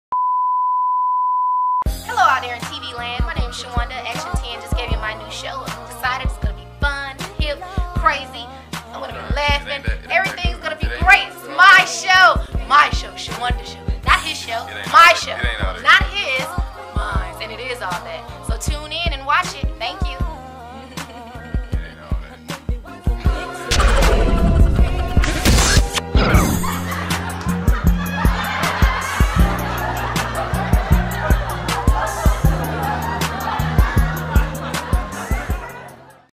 Shawanda Show Promo
Coming Soon to Daniel Kahn Nelson TV!!  A new sitcom starring the emotionally distraught comedic antics of everyones favorite girl, Shawanda Hosley.